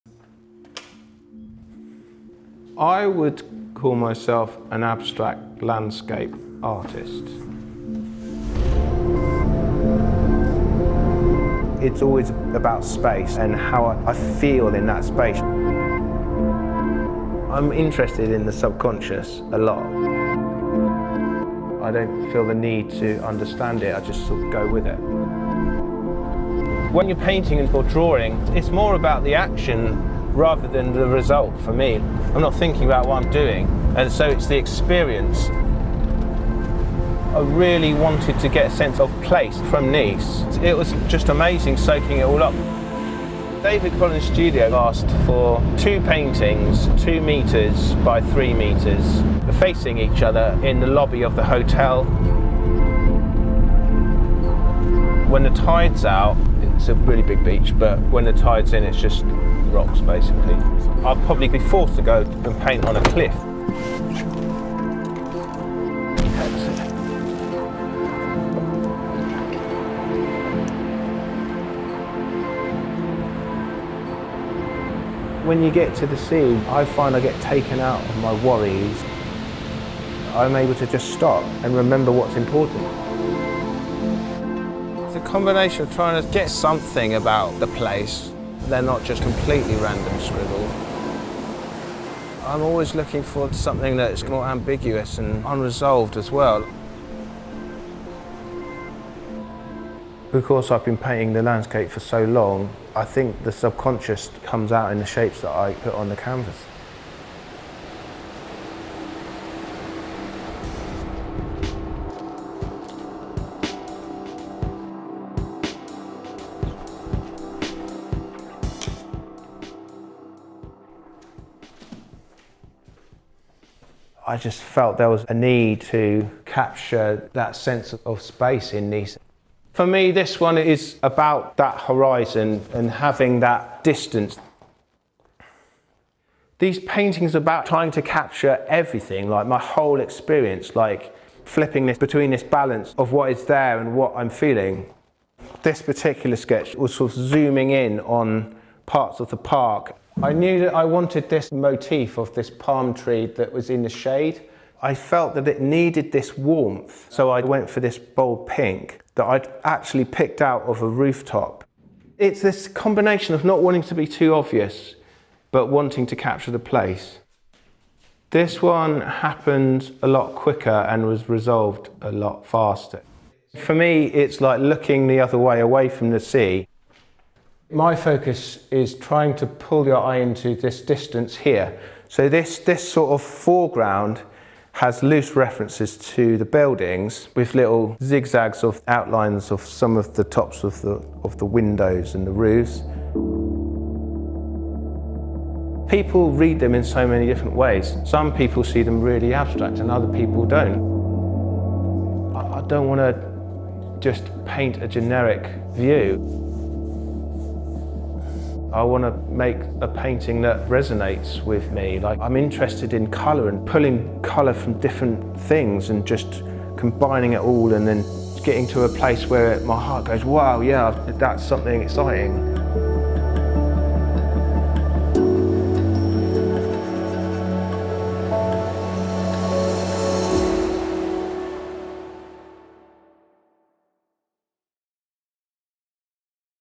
Cornish Artist Saul Cathcart's Creative Process For Anantara Nice Plaza Hotel
Behind the scenes look at the redesign of Anantara Nice Plaza, by David Collins Studio. Chief Creative Officer Simon Rawlings envisioned a lobby space with 'windows' onto the outside world and the hotel's iconic location. Cornish artist Saul Cathcart was commissioned to create a pair of large-scale works, depicting the two key views from the hotel rooftop restaurant and bar. The park and sea to one side, and the mountains and rooftops behind are realised in vivid and unexpected colours. Saul's work hangs at either end of the grand lobby and bring movement to the otherwise serene space.